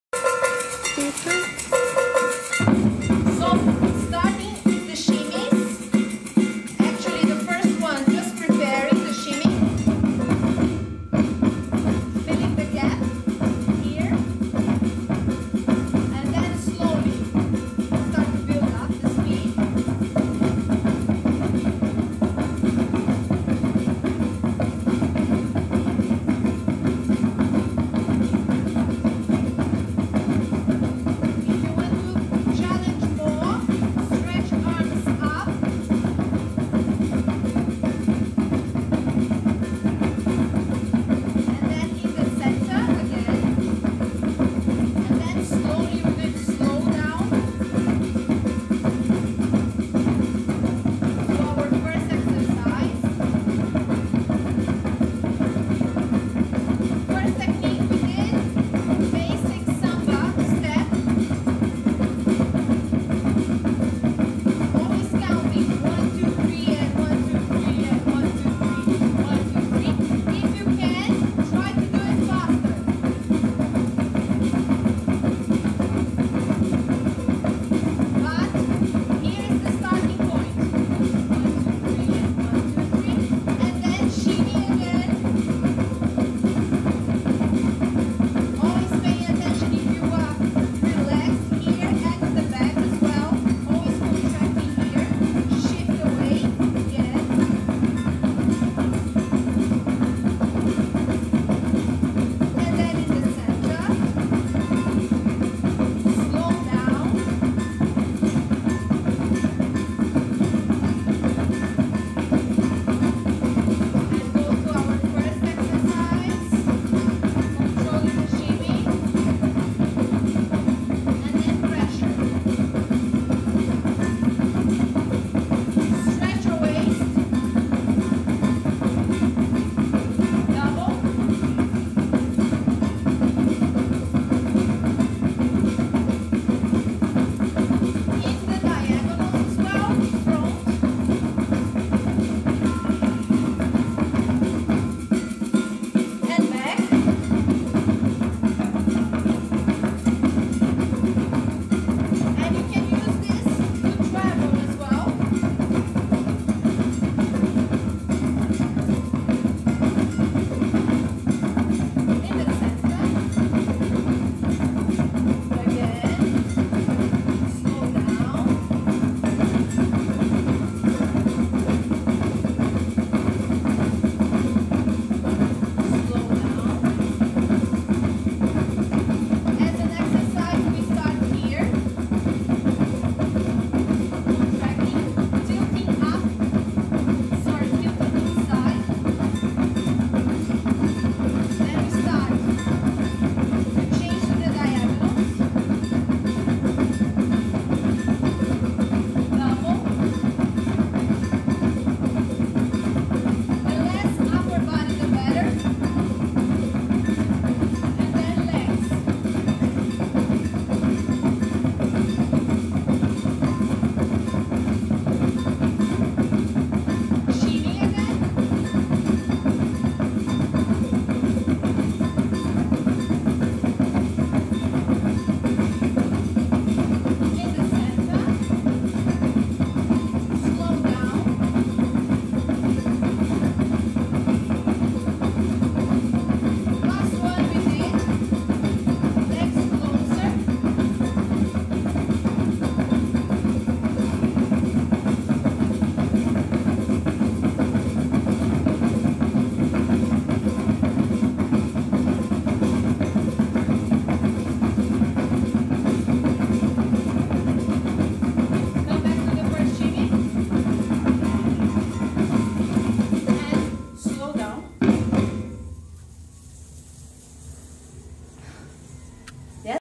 Elis Pinheiro Workshop - Hips Techniques + Brazilian Dance Influences
Some hip techniques and shimmies + some brazilian flavour. Elis Pinheiro workshop in Coventry, Midlands, United Kingdom. March 2013.